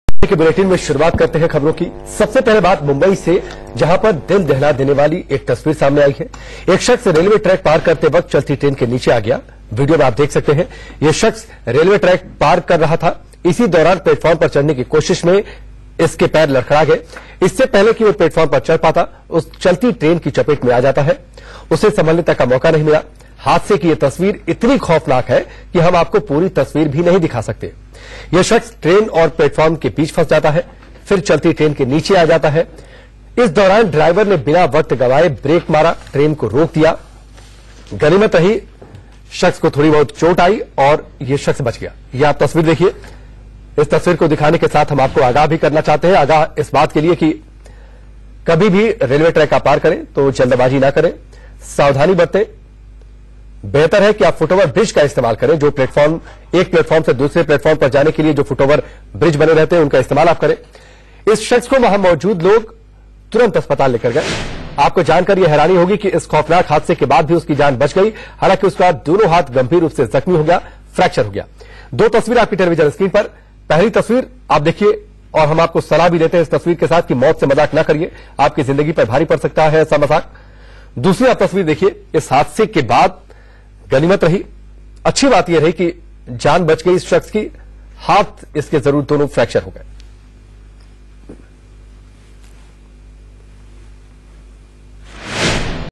मुंबई में ट्रेन और प्लेटफॉर्म के बीच फंसा शख्स, मौत के मुंह से निकला बाहर
मुंबई से दिल दहला देने वाला वीडियो सामने आया है. एक शख्स रेलवे ट्रैक पार करते वक्त चलती ट्रेन के नीचे आ गया. ये शख्स रेलवे ट्रैक पार कार प्लैटफॉर्म पर चढ़ने की कोशिश कर रहा है. इससे पहले की वो प्लेटफॉर्म पर चढ पाता, उसके चलती ट्रेन की चपेट में आ जाता है. उसे संभलने तक का मौका नहीं मिला. ये शख्स ट्रेन और प्लेटफॉर्म के बीच फंस जाता है और फिर चलती ट्रेन के नीचे आ जाता है. ड्राइवर ने बिना वक्त गवांए ब्रेक मारकर ट्रेन रोक दी.